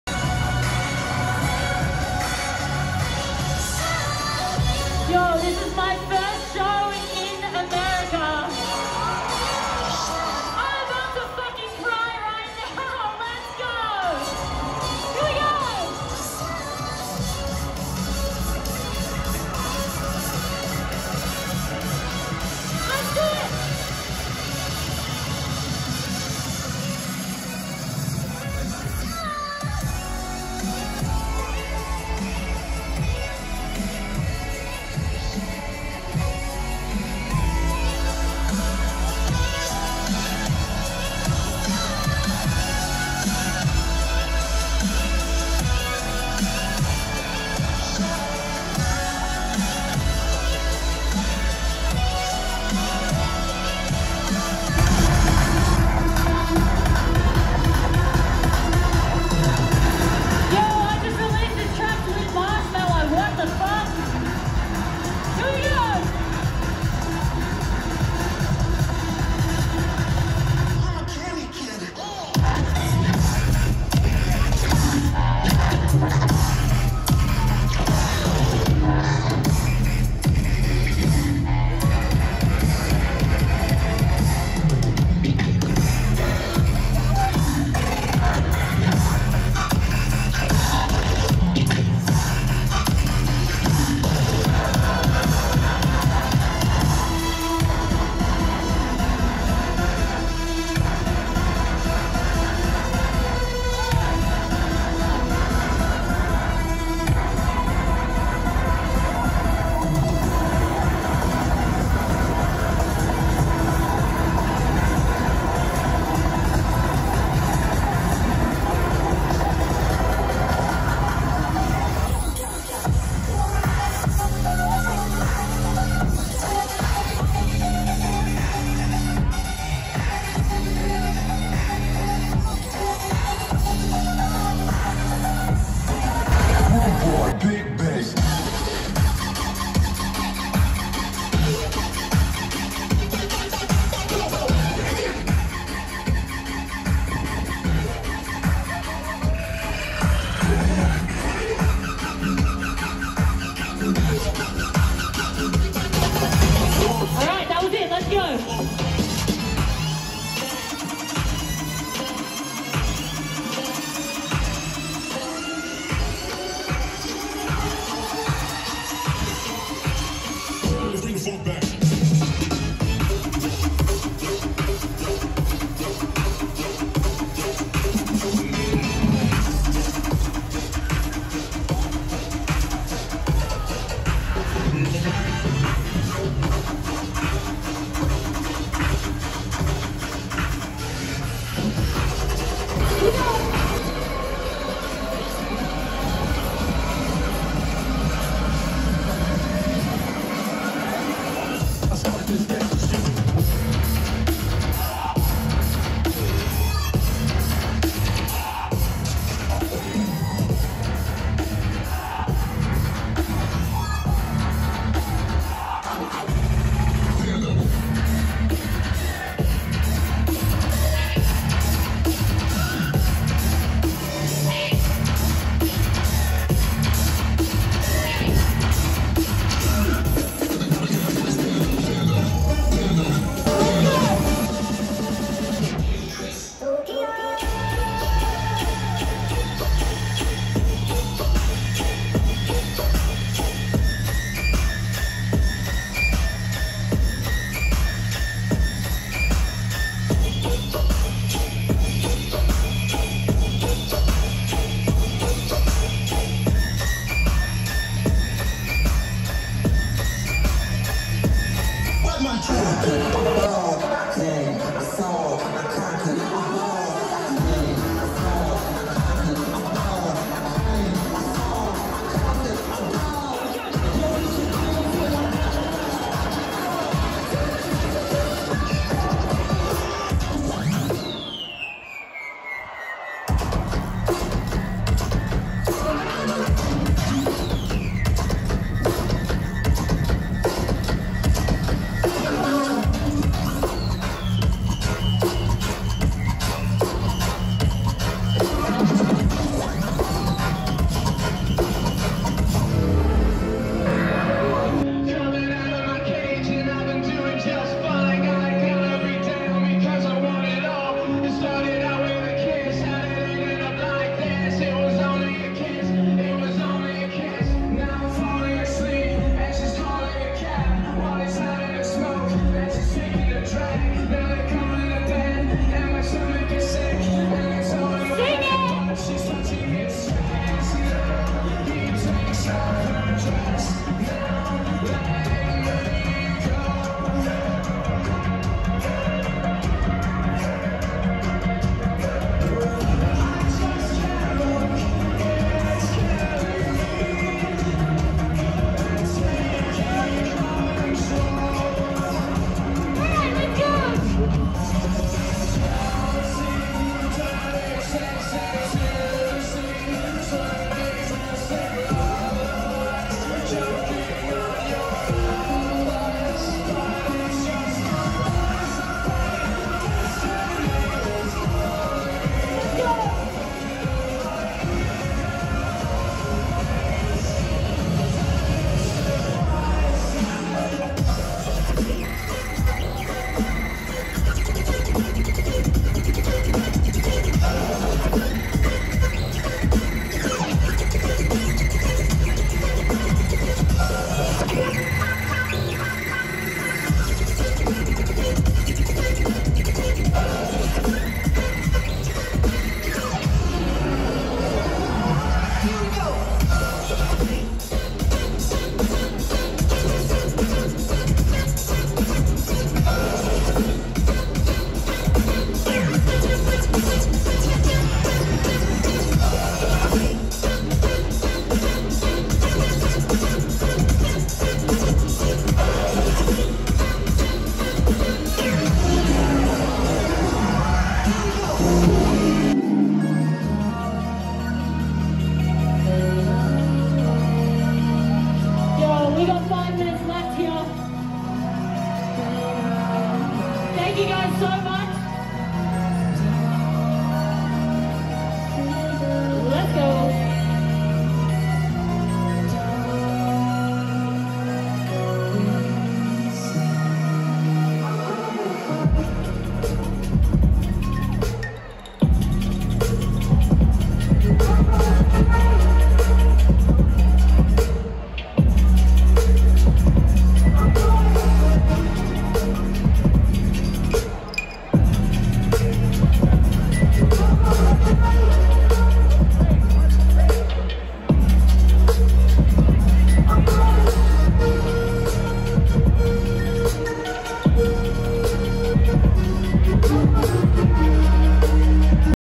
SIPPY Red Rocks Debut
The Bass Queen took Denver by storm (yes it rained) this weekend, and threw down one of the best sets of the weekend. Hair bouncing, smiles, and even some tears SIPPY delivered all the feels. Very lucky to have experienced this and wanted to share it with those who love special talent and good music. Until next time, enjoy!